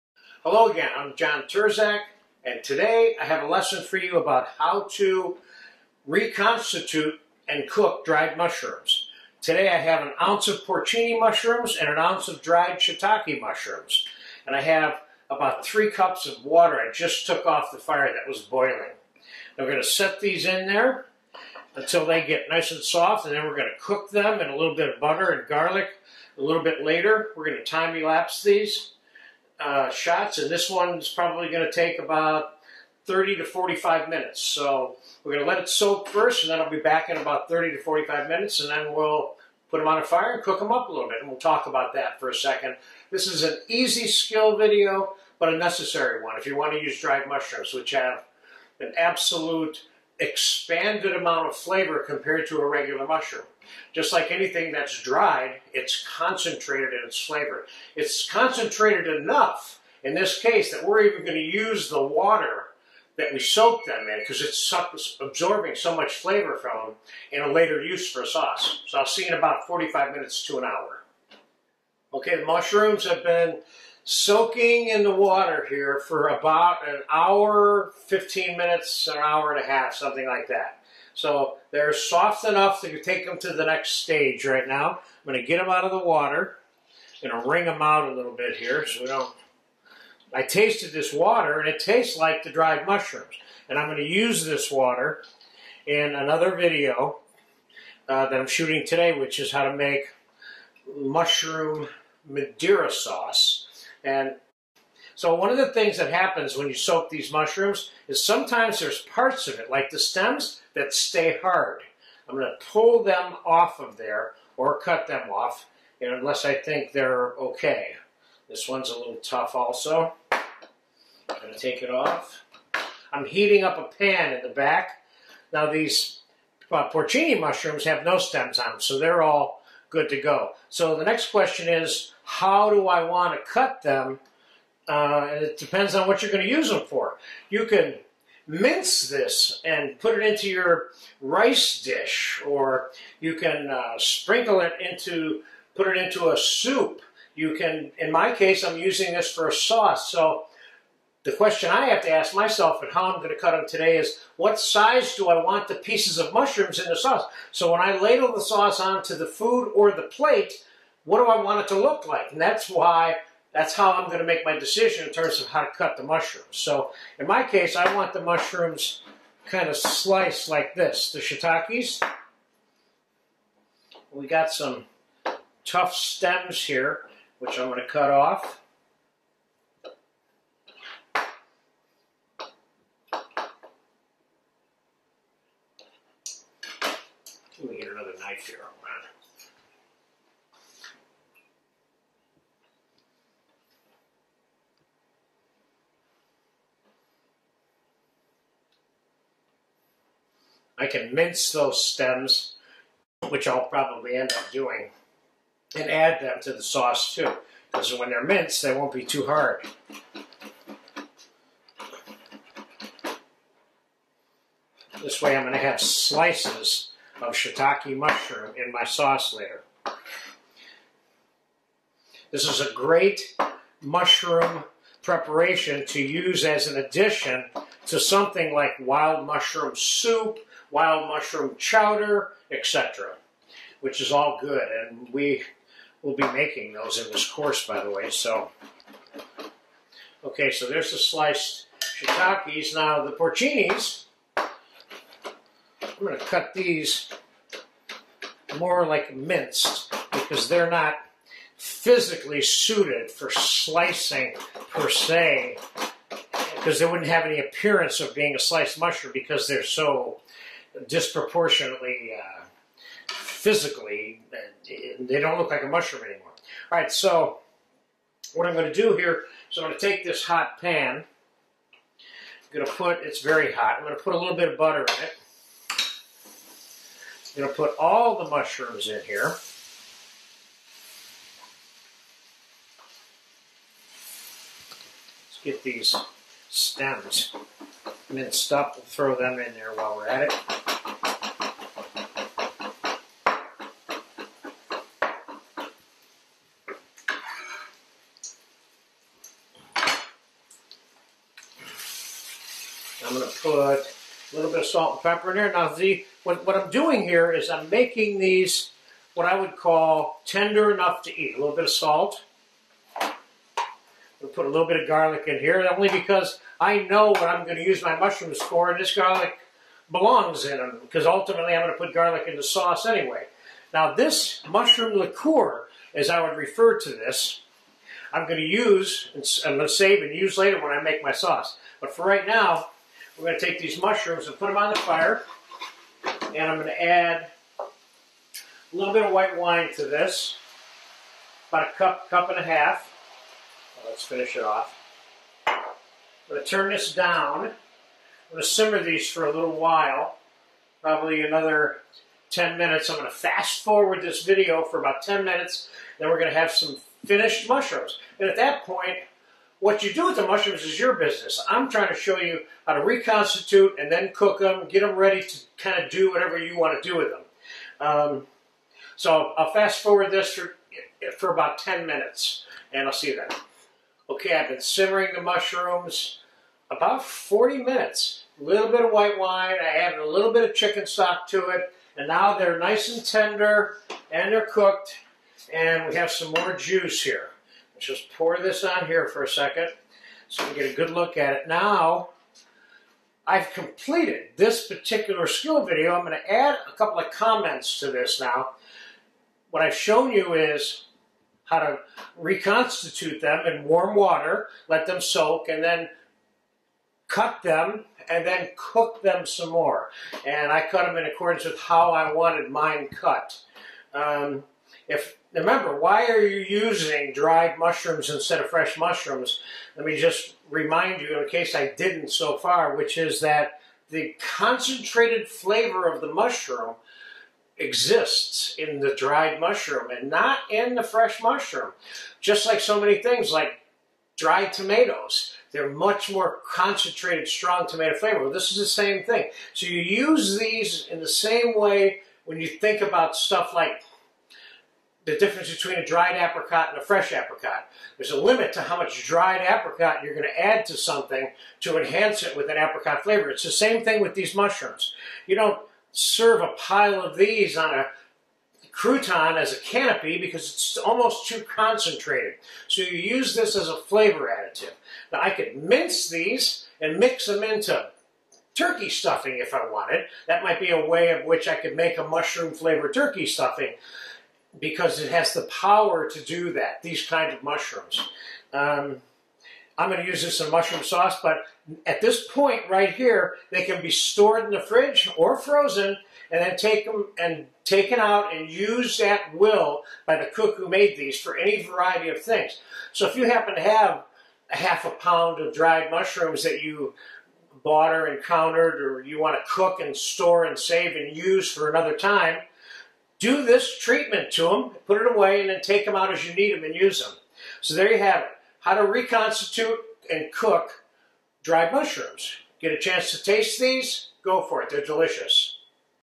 Vintage - 2016 - Dried Mushroom Prep/Shiitake & Porcini - RV KITCHEN
DESCRIPTION
In this video you'll see dried shiitake & porcini  mushrooms reconstituted, cleaned, cut, cooked in white wine, chicken stock & garlic, seasoned and stored for a later use.
You'll also hear a lot of good advice about how to handle these and some of the smart ways to use these kinds of mushrooms in your cooking.
This necessary skill can provide many concentrated mushroom flavors that might otherwise be lost, if not done correctly. In this case I also use the warm water that the dried mushrooms were reconstituted in as part of a sauce making flavor additive. This reconstituting process can also be applied to other dried foods in whole or in part. Remember, dried foods take on a concentrated flavor once they are dehydrated. Sometimes certain fruits or vegetables should only be partially rehydrated for optimum effects for certain preparations.

HIGHLIGHTS
Yield: 2 full cups
Hands on Preparation Time: 10-15 minutes
Total Preparation Duration: about 2 hours

INGREDIENTS
2 ounces dried mushrooms
3-4 cups of boiling water
2-3 ounces whole unsalted butter
1-2 tablespoons fresh chopped garlic
1-1-1/2 cups dry white wine
salt & fresh ground pepper to taste

METHOD
Begin by soaking the mushrooms in the water that was just boiled for 45 minutes to an hour. Remove them from the water you soaked them in and squeeze them a little in preparation for cutting them on your cutting board. Save the water because you will be able to use it in other preparations to add mushroom flavor.
Trim, clean and/or slice the mushrooms in accordance with their intended use. Now add them to a hot saute pan with the whole butter, garlic, a little salt & pepper and toss them around a little. Now add the white wine and return the saute pan to the fire on a low simmer. Keep in mind that you may use different wines such as white Zinfandel, Madeira, Ports, Sherries or even red wines. Simmer the mushrooms for 30 minutes to an hour or until you think they are tender enough to eat. Remove them from the heat and allow them to cool.
This preparation is now completed.

COMMON MISTAKES
Not cooking the mushrooms until they are tender enough to eat.

STORAGE SPECIFICATIONS
You may store the cooked reconstituted mushrooms covered in the fridge for up to two weeks without hesitation. You may also freeze these mushrooms for 4-6 weeks without hesitation.

CHEF'S NOTES
This necessary skill can provide many concentrated mushroom flavors that might otherwise be lost, if not done correctly. In this case I also use the warm water that the dried mushrooms were reconstituted in as part of a sauce making flavor additive. This reconstituting process can also be applied to other dried foods in whole or in part. Remember, dried foods take on a concentrated flavor once they are dehydrated. Sometimes certain fruits or vegetables should only be partially reconstituted for optimum effects for certain preparations.

LEFTOVERS
These mushrooms can be added to rice's, soups, stews, stuffing's, pastas etc...

SPECIAL INGREDIENTS I MAY HAVE USED IN THIS VIDEO:
My Chicken Stock Video: